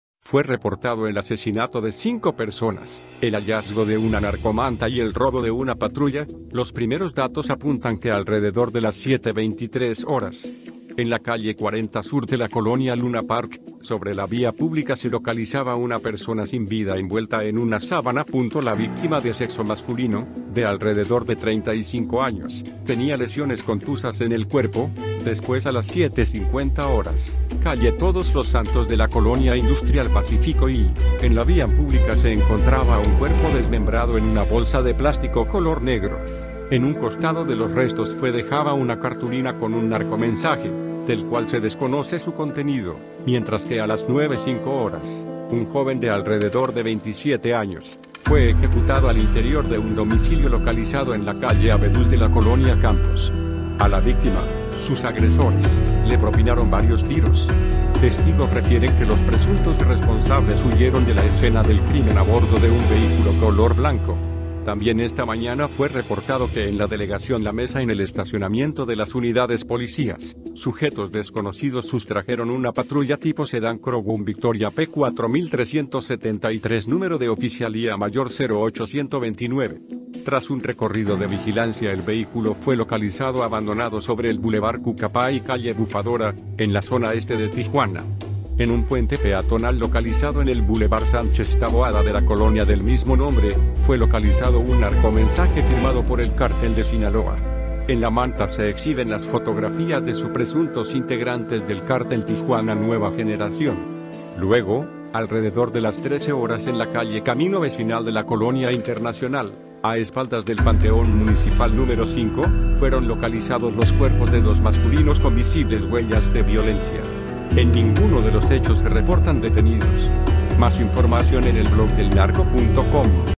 Confirman cinco ejecutados, cuelgan narcomanta y roban una patrulla este domingo
Fue reportado el asesinato de cinco personas, el hallazgo de una narcomanta y el robo de una patrulla,  Los primeros datos apuntan que alrededor de las 07:23 horas.

En la calle 40 Sur de la colonia Luna Park, sobre la vía pública se localizaba una persona sin vida envuelta en una sábana.La víctima de sexo masculino, de alrededor de 35 años, tenía lesiones contusas en el cuerpo,  Después a las 07:50 horas, calle Todos los Santos de la colonia Industrial Pacifico II, en la vía pública se encontraba un cuerpo desmembrado en una bolsa de plástico color negro.